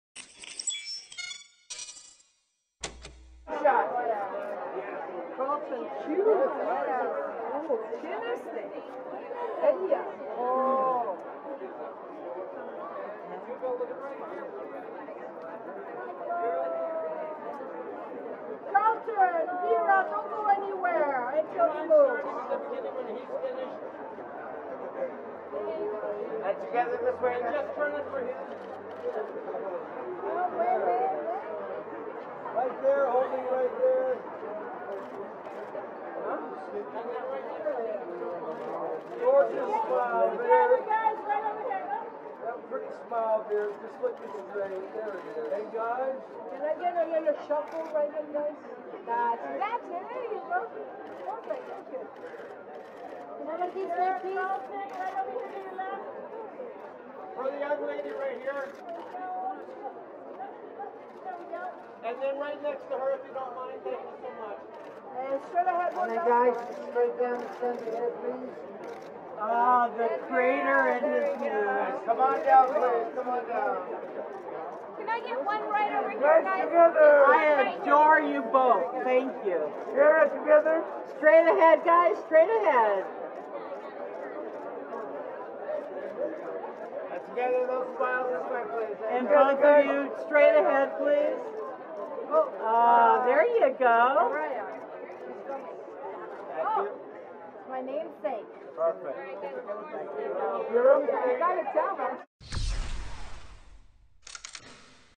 Carlton Cuse and Vera Farmiga “Five Days at Memorial” Red Carpet Premiere Arrivals | Apple Original
Broll footage: Showrunner Carlton Cuse and actress Vera Farmiga attend the red carpet premiere of the Apple Original series “Five Days at Memorial” held at the Directors Guild of America Theater in Los Angeles, California USA on Monday August 8th, 2022. “Five Days at Memorial” premieres globally on Apple TV+ on August 12, 2022.